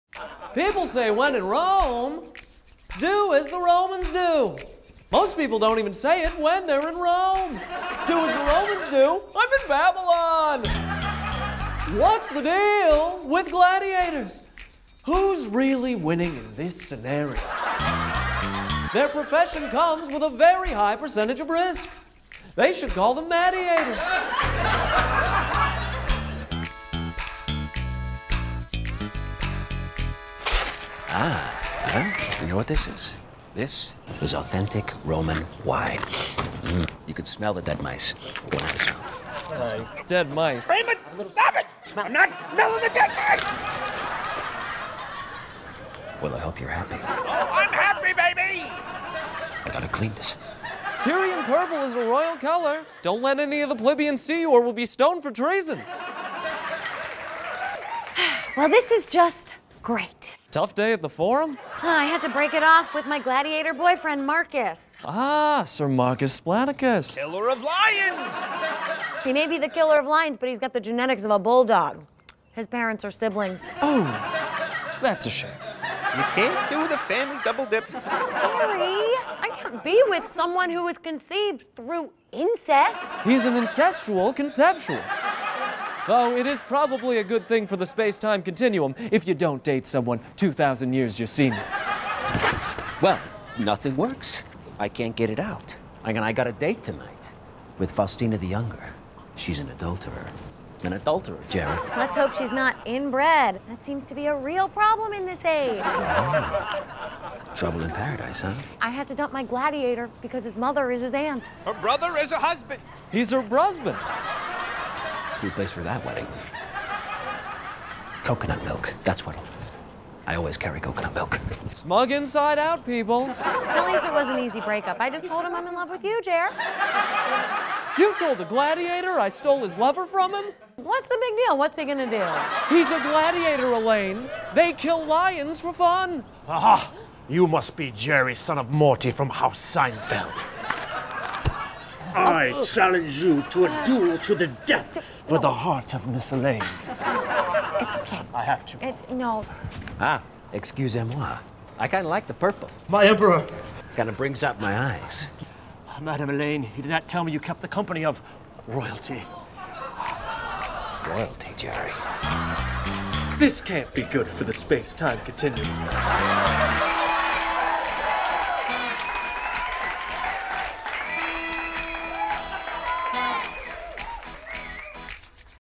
Timefeld 2 - Ancient Rome
Here it is folks! Timefeld Episode 2: Ancient Rome! Starring me as Jerry, Brock Baker as Costanza, Luke Cook as Kramer, and Madeline Walter as Elaine! Also featuring Christopher Robert Smith as our hunky gladiator. Directed by Chuck Kovalik, sound engineer/editing by Marcos Butron Jr, and music by the toast of Second City Los Angeles, Hughie Stone Fish!  Costumes by Shay Gilbert and other stuff by Brian Kessler!